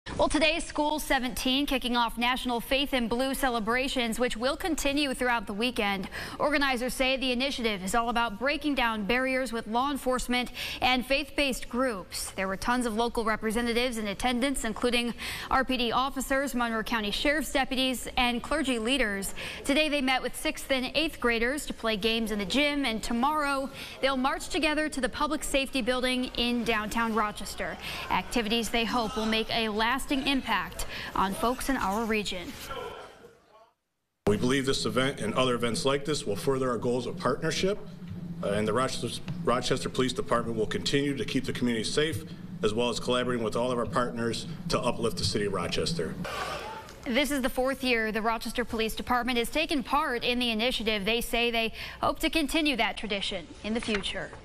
Faith and Blue Weekend begins in Rochester
Today RCSD School No. 17 kicked off National Faith and Blue celebrations, which will continue throughout the weekend.